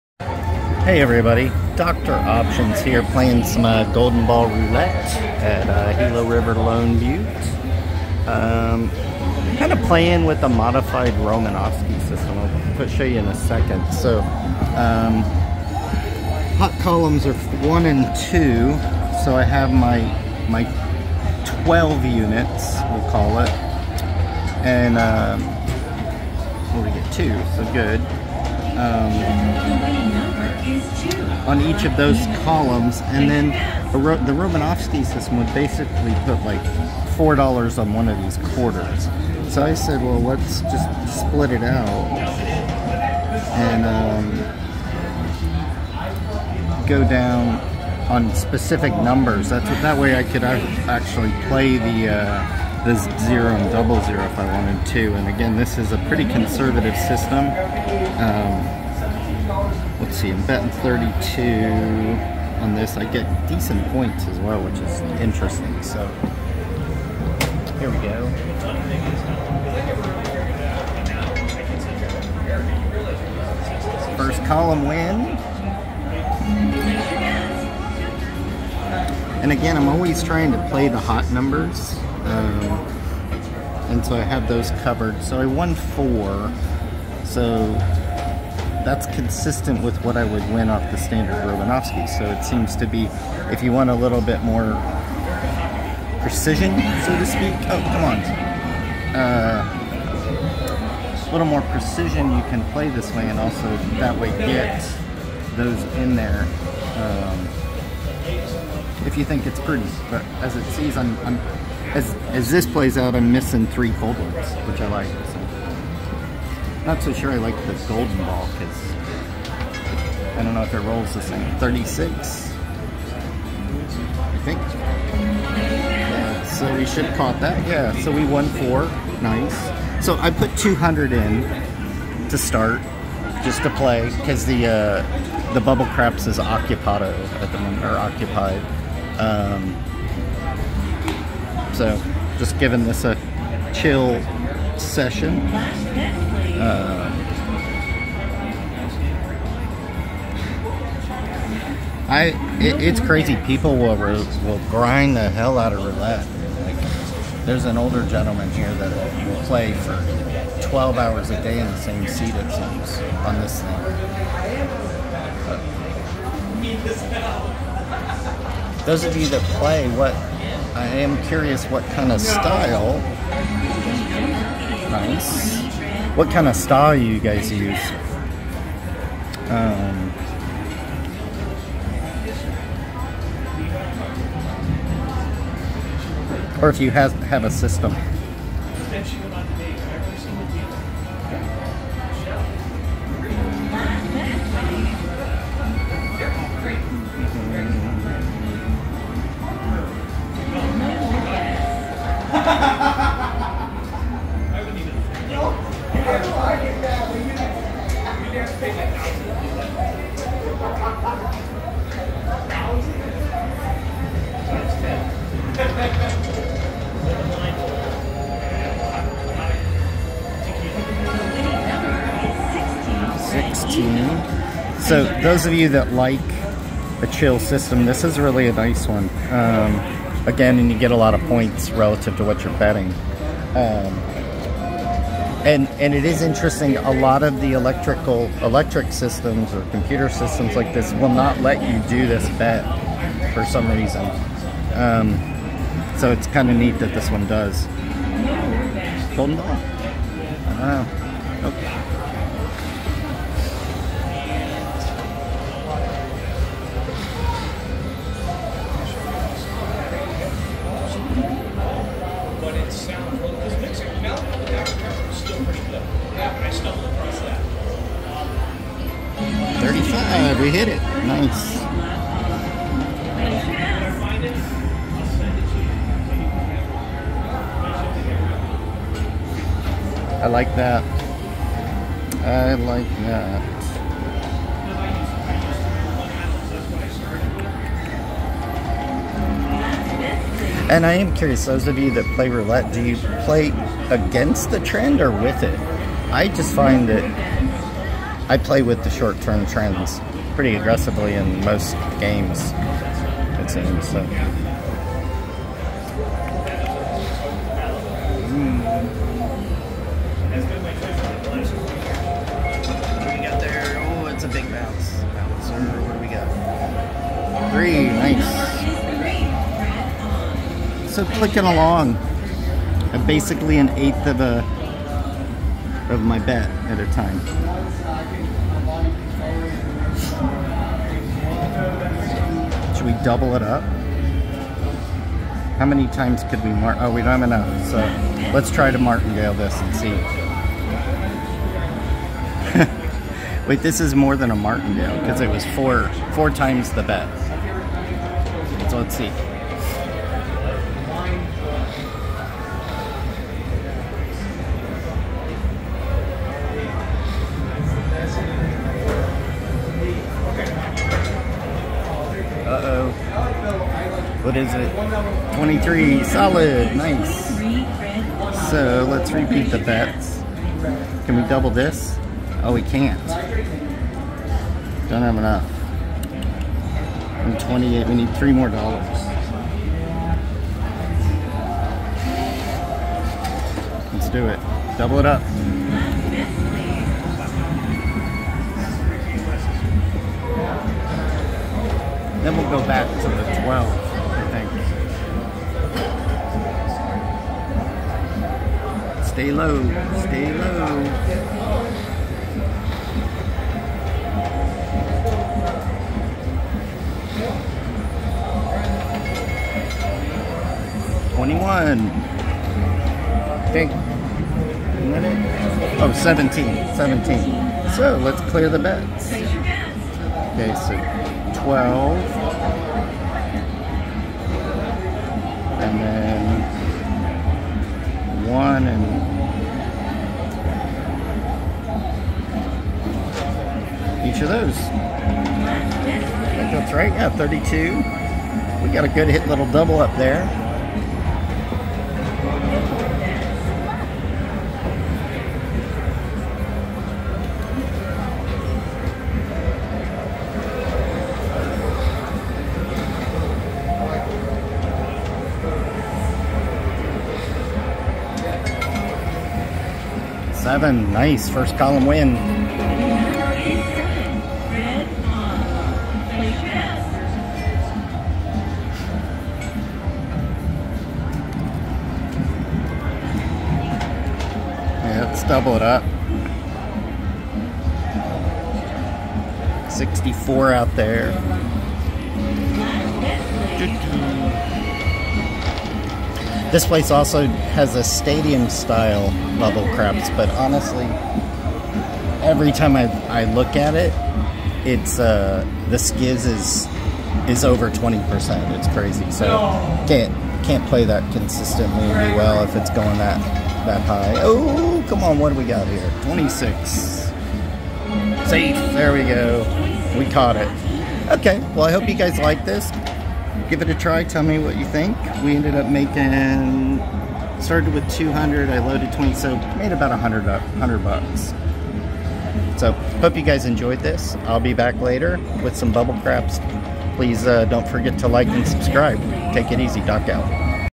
with Modified Romanovsky System
To prep for my next rousing round of the @dr.options Bubble Craps $2,500.00 "MAX BET CHALLENGE" Rolls #76-80, I took a little break to chill and hit some Golden Ball Roulette, utilizing a Modified Romanovsky System of my own creation (I think) to run through some lower bets to see how it holds up @GilaRiverResortsCasinos.

In this system, 12 units on each 1/3 Column and 1 unit on 8 numbers of your choice. Please let me know your thoughts.

Doc